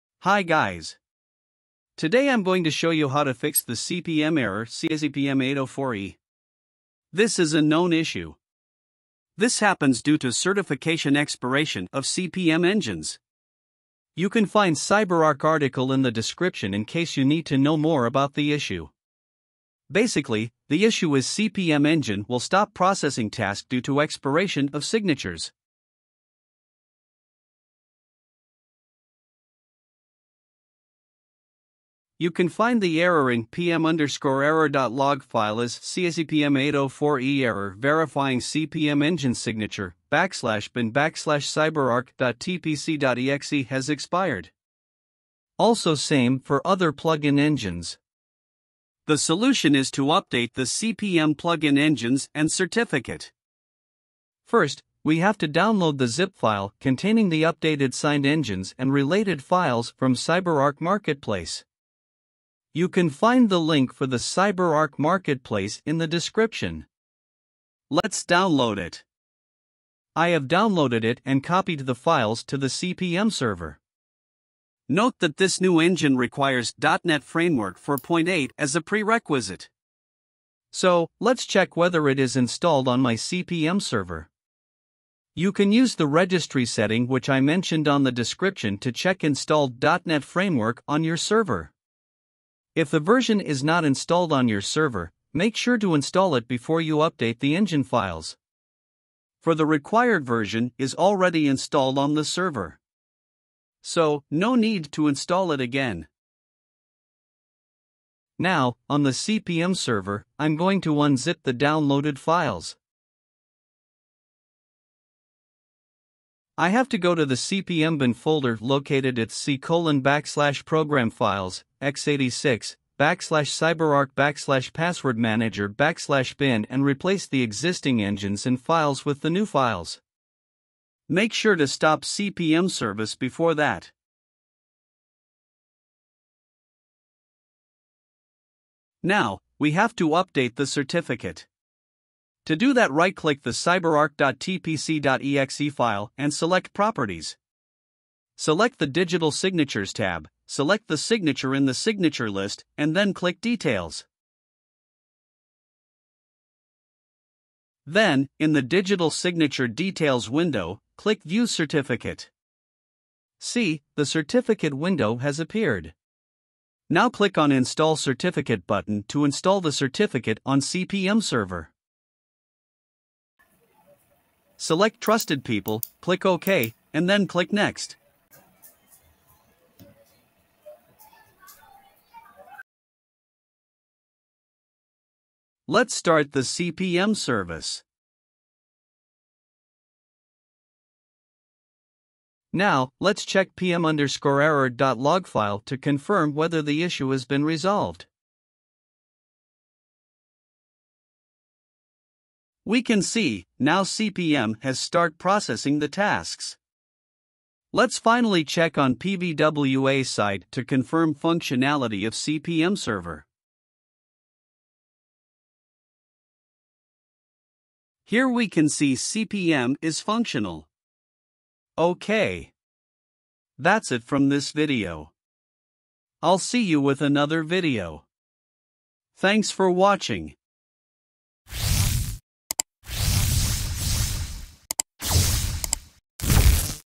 CPM engine certificate expiration notification | CACPM804E Error verifying CPM engine signature
CyberArk Article
Registry path to check .NET version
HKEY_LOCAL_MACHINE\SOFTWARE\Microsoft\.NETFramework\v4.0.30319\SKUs